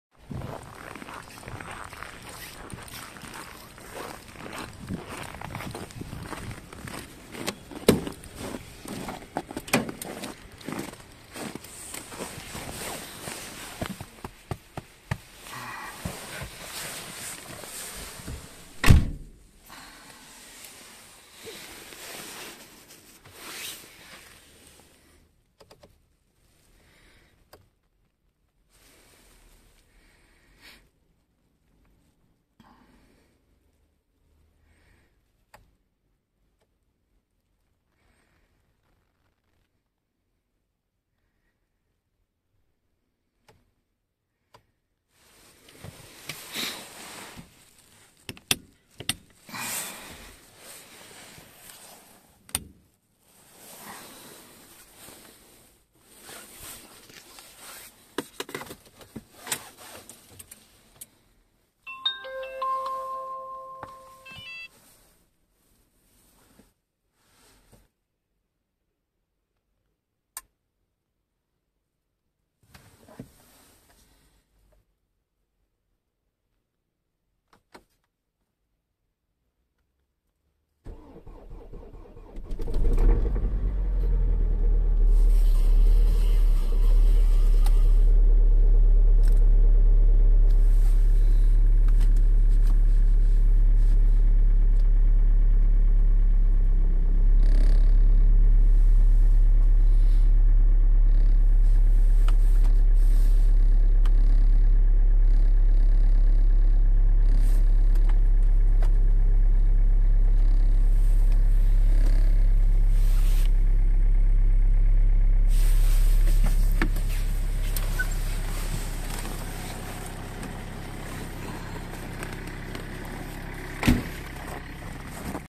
Запуск в мороз Renault Grand Scenic 3 1.9dci Socar Diesel Nano
Думал что в -19 это не реально , но залил арктический дизель Socar и запустилась очень даже легко как для такой температуры .